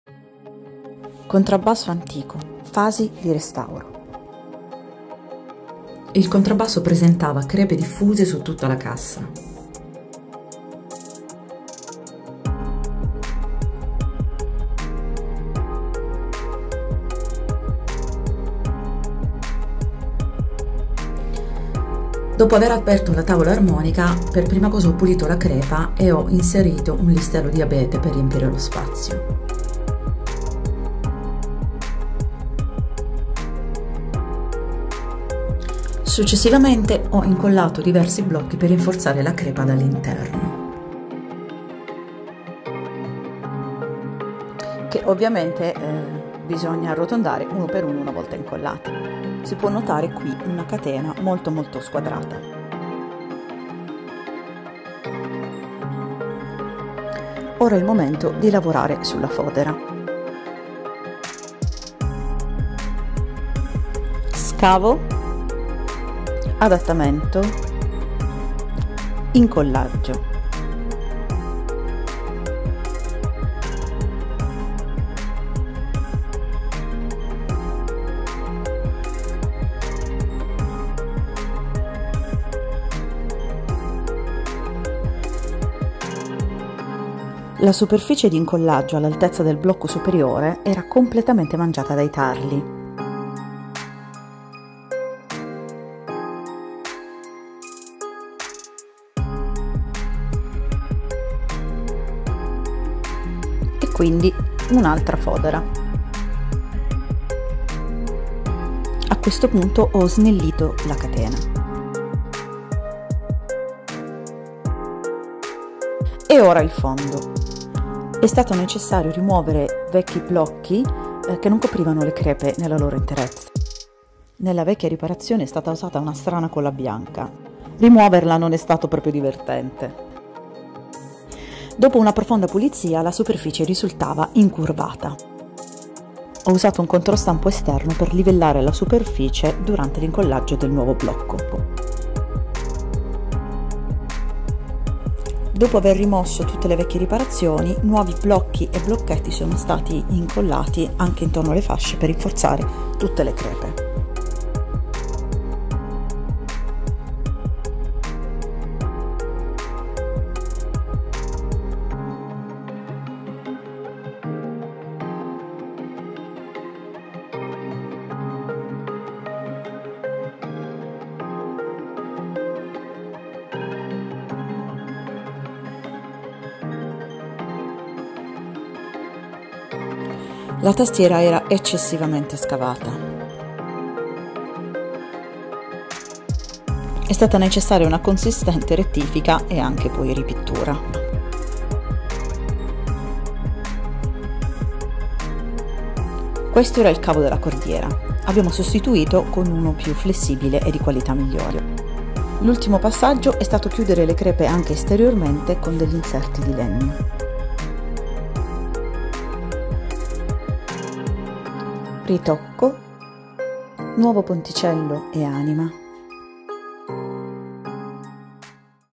Double Bass Restore. Screen description ENG, Audio ITA
Double bass restoration. One of the biggest project I ever worked on. I started in november last year and recently finished. All of that wouldn't be possible without the help and mentoring of my dear friend and great restorer Melle Wondergem in Amsterdam.

SOCIALS & WEBSITE - follow me for daily contents 👇🎻